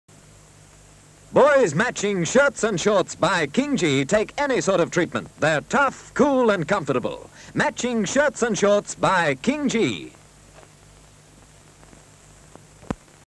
King Gee Shorts 1960 TV commercial
1960 Australian 10sec TV ad made by Avondale Studios (PN117b) for Arthur Smythe (KG14)